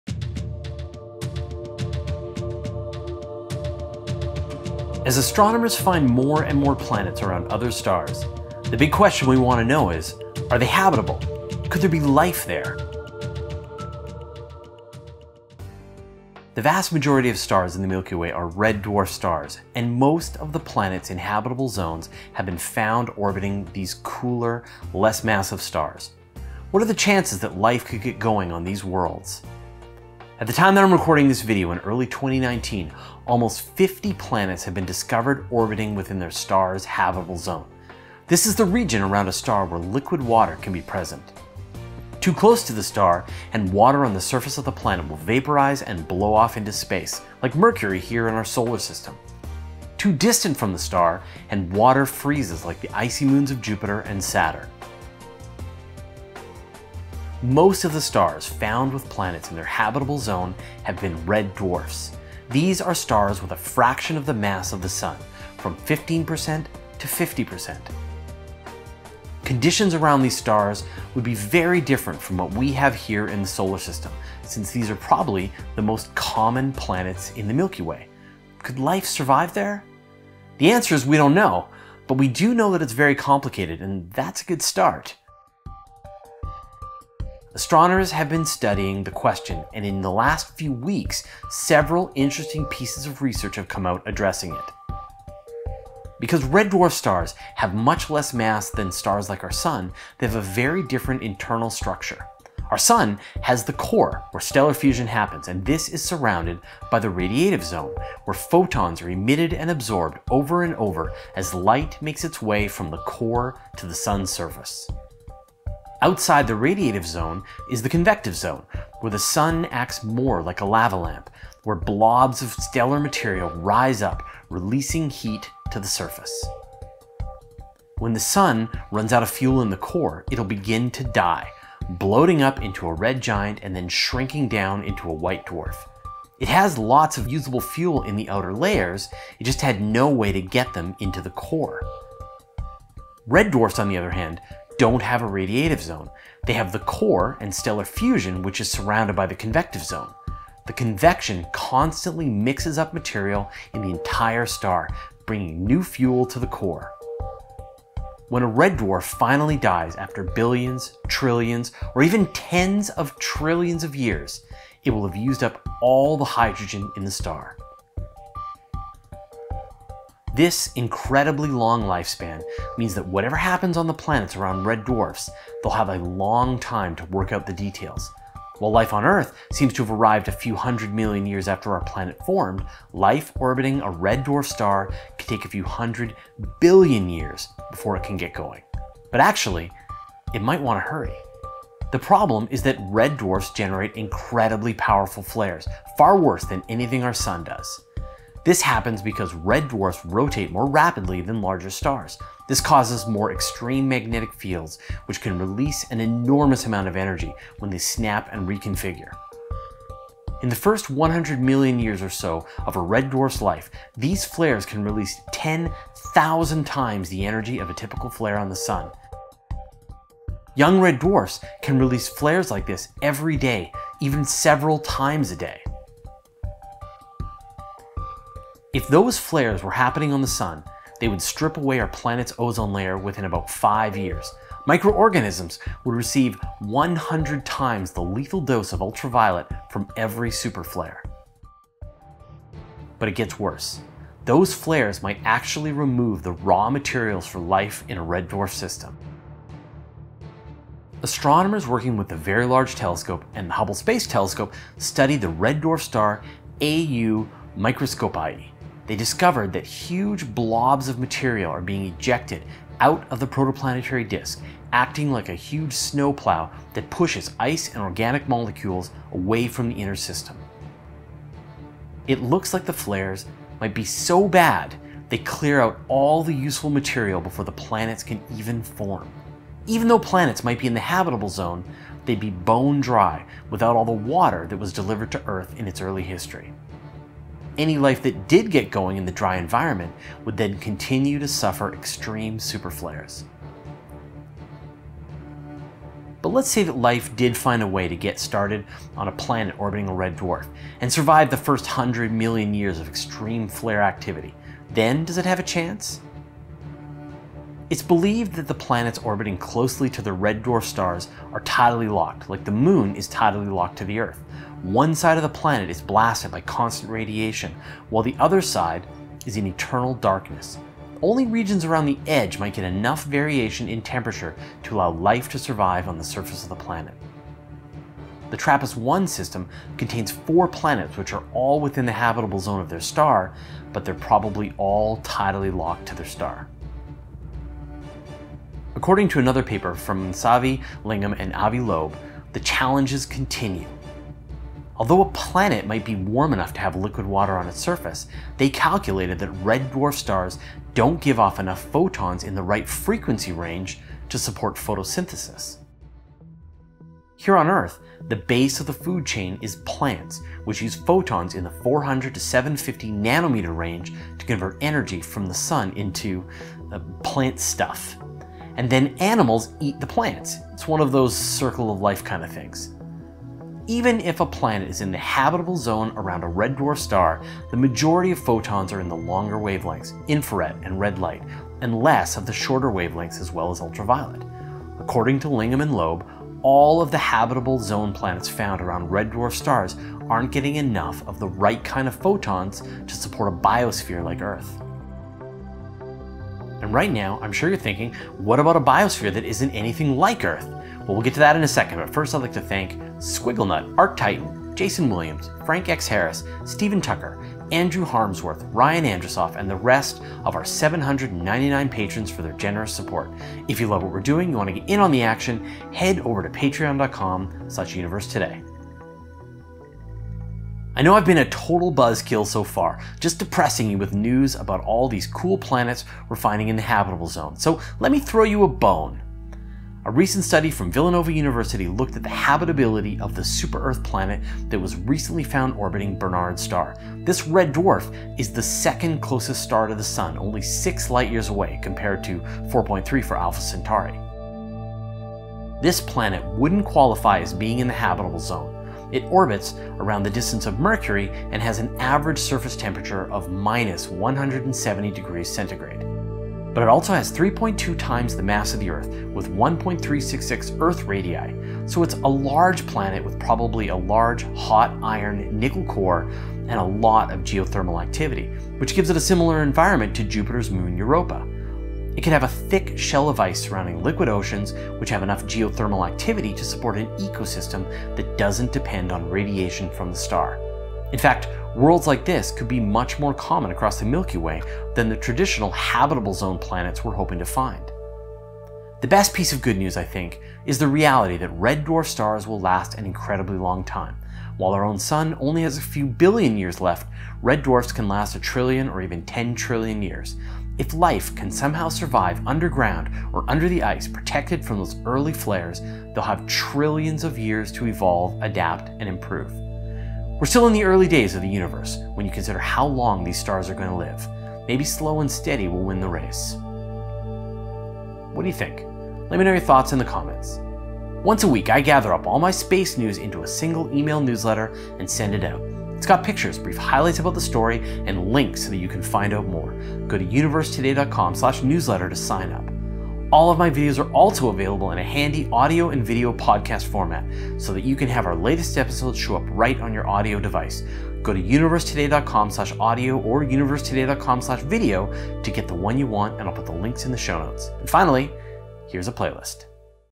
Can There Be Life On Planets Around Red Dwarf Stars?
As astronomers find more and more planets around other stars, the big question we want to know is: are they habitable, could there be life there?

The vast majority of stars in the Milky Way are red dwarf stars, and most of the planets in habitable zones have been found orbiting these cooler, less massive stars. What are the chances that life could get going on these worlds?

Chloe Cain - Instagram: @chloegwen2001